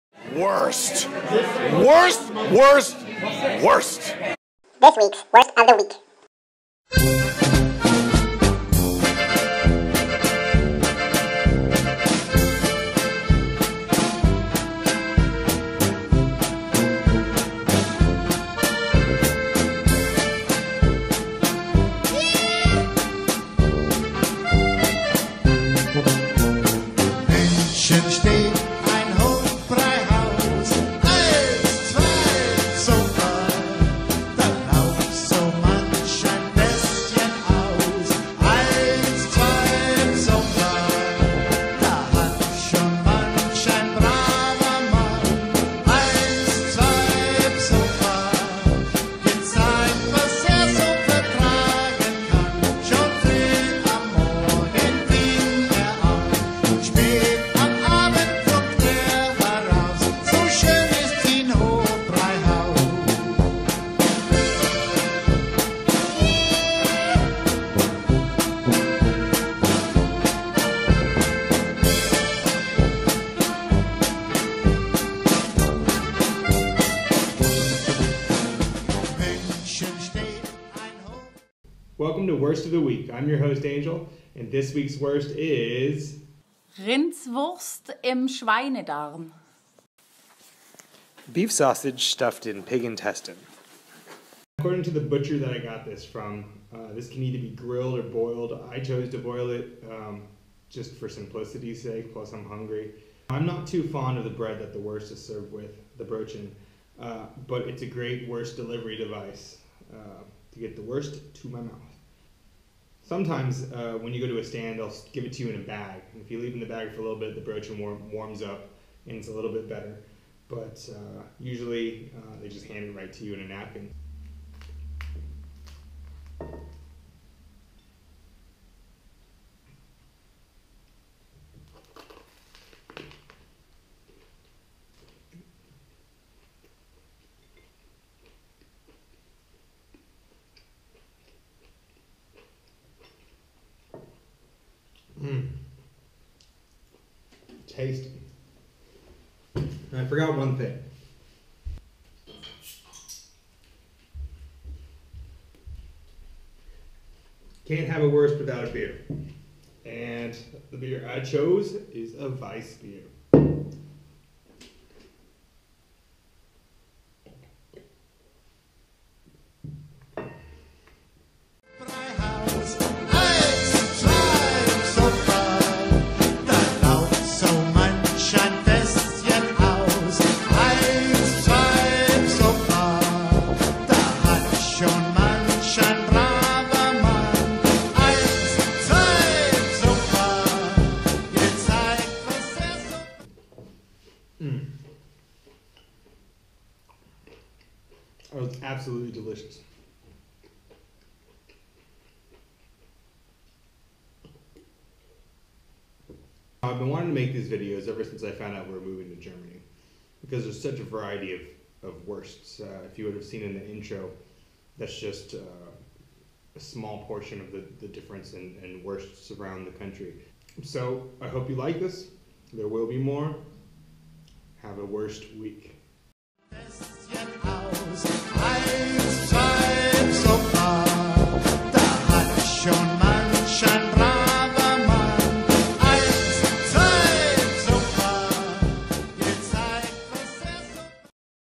Wurst Episode 1 Revised
Episode 1 (revised with commentary) of Wurst of the Week; Rindswurst im Schweinedarm (beef sausage stuffed in pig intestine). The butcher said this is one of the most common wursts in Germany and can be boiled or grilled.